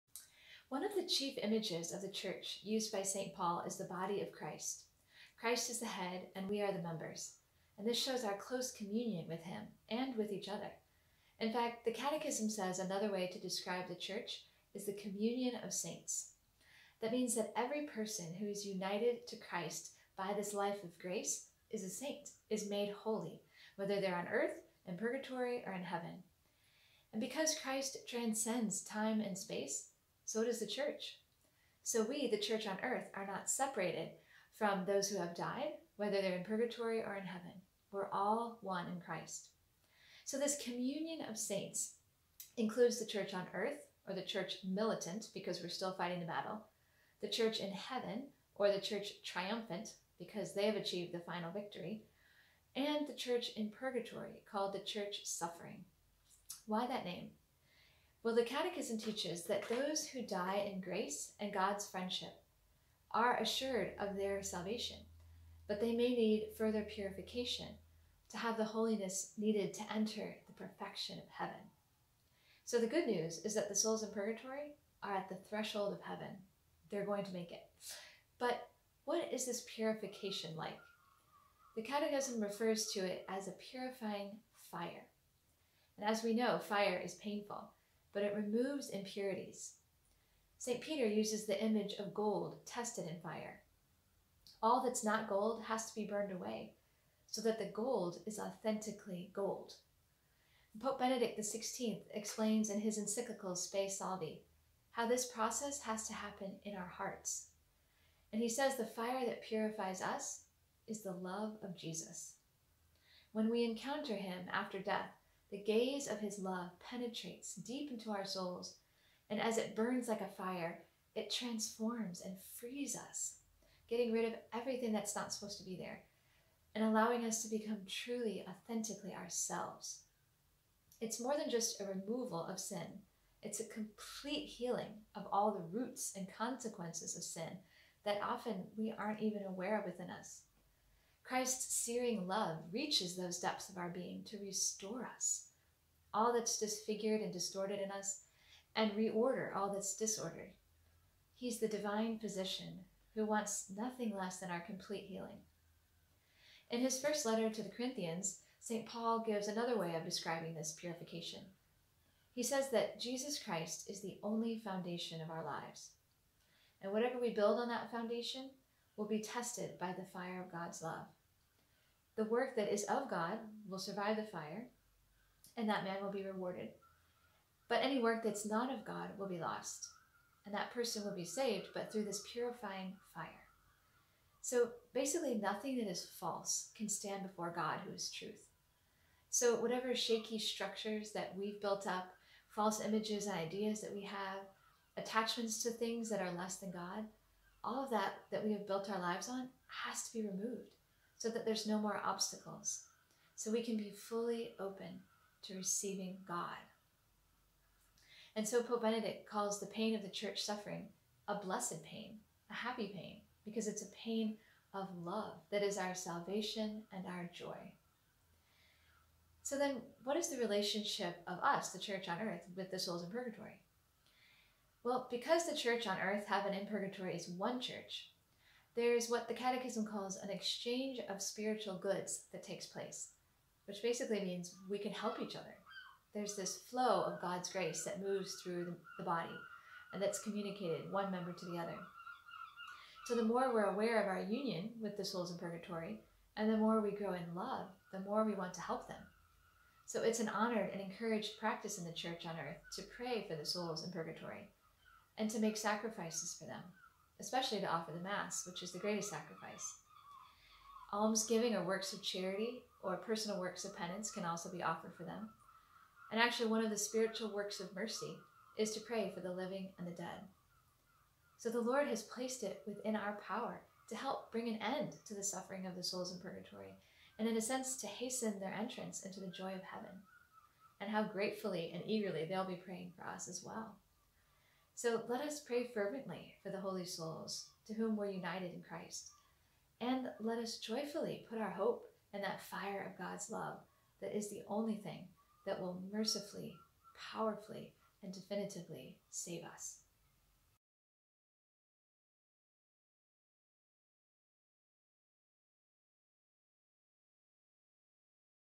Church in Purgatory
This Five Minute Ambo focuses on the Church in Purgatory, given by Sr. Mary Gianna.

Five Minute Ambo is a five-minute catechesis series produced by the Ogilvie Centre.